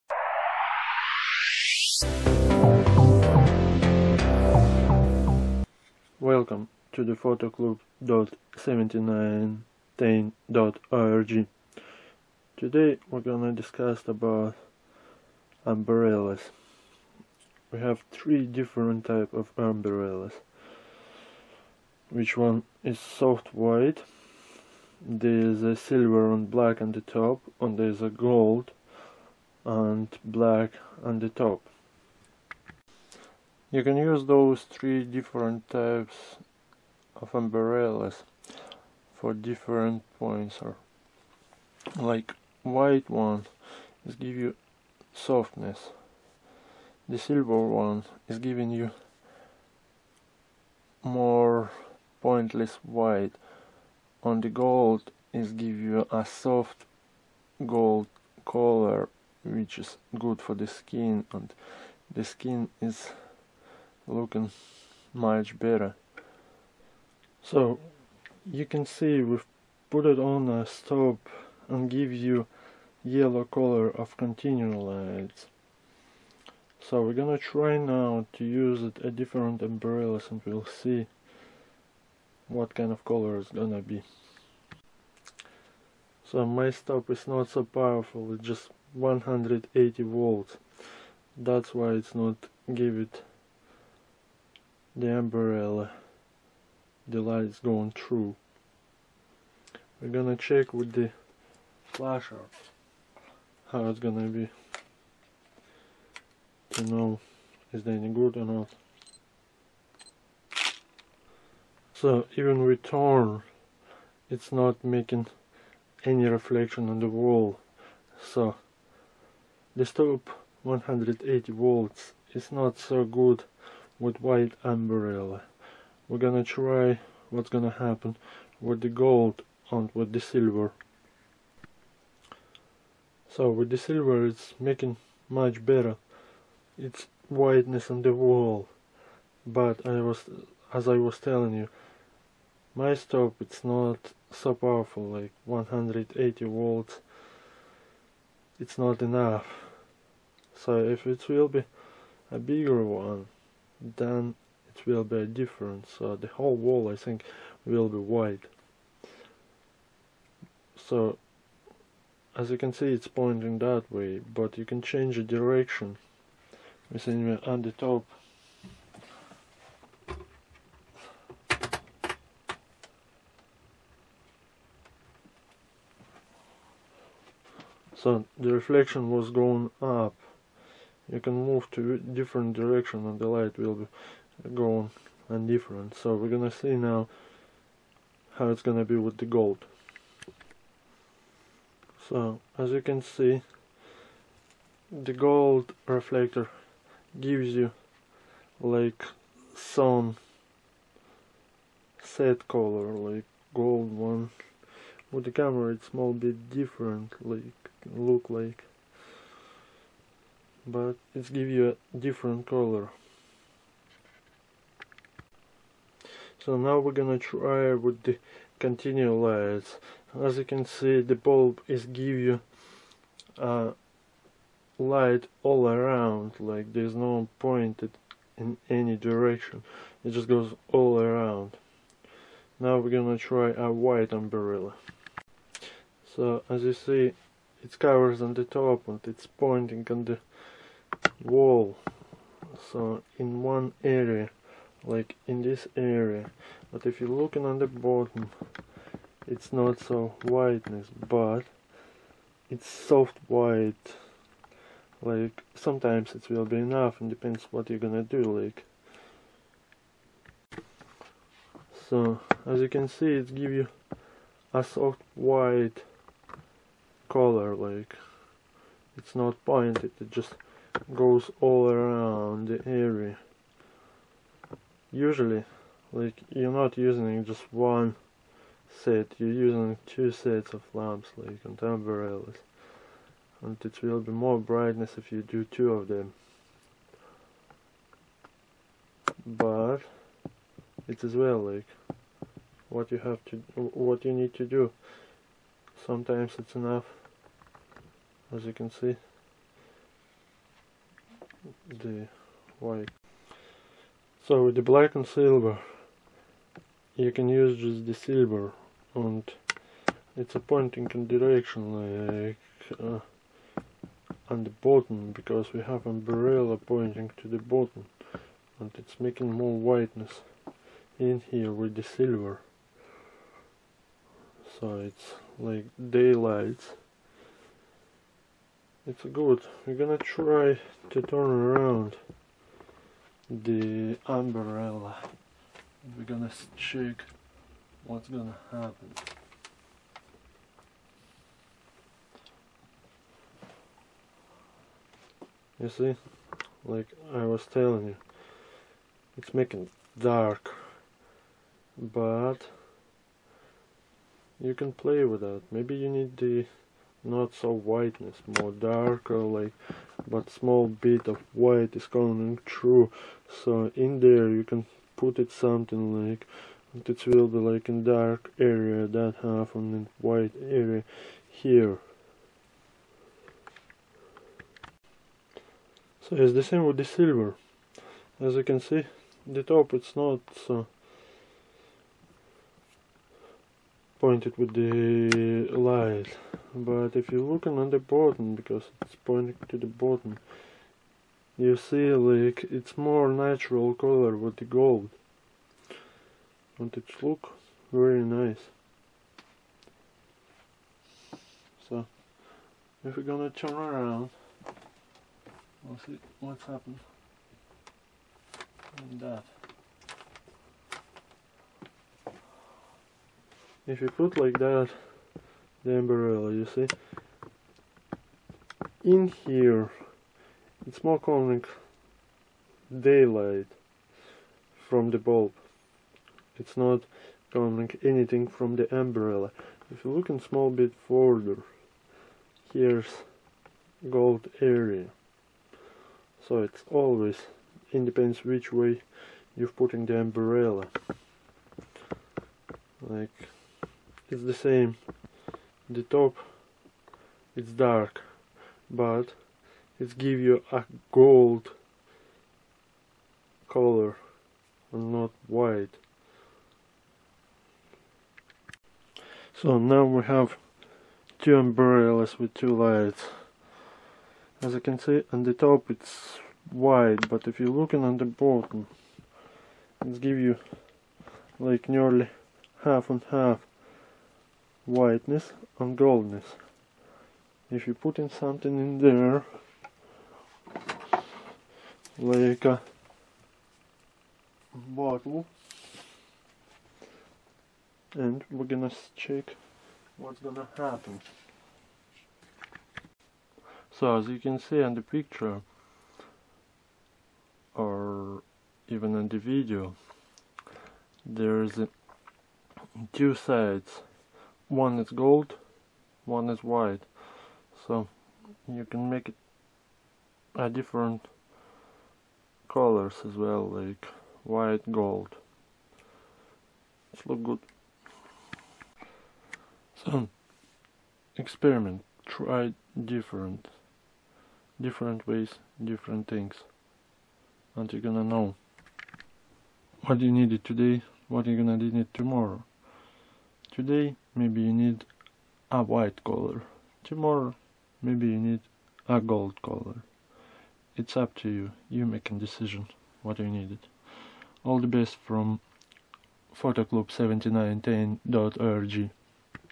Umbrella: Reflective Photo Studio Umbrella Gold, Silver, White
This black/gold and White umbrellas serves as a quick set up reflective umbrella, acting as a quick modifier for your light. The gold reflective material give your light a warm glow, adding a tough of sun to your photographs. Available in various sizes to direct your light as needed, this umbrellas is great for in studio and on location photo shoots.